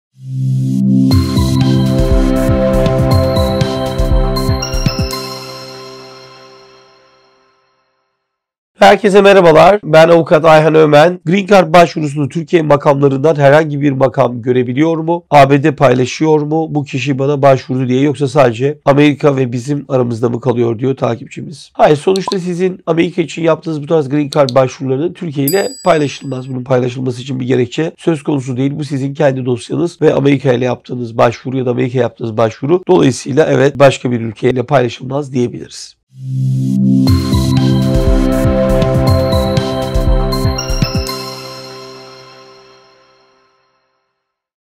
GREEN CARD BAŞVURUMUZUN BİLGİSİ TÜRK MAKAMLARIYLA PAYLAŞILIYOR MU ?
Soru: Green Card başvurusunu Türkiye makamlarından herhangi bir makam görebiliyor mu ? ABD paylaşıyor mu, bu kişi bana başvurdu diye yoksa sadece Amerika ve bizim aramızda mı kalıyor ?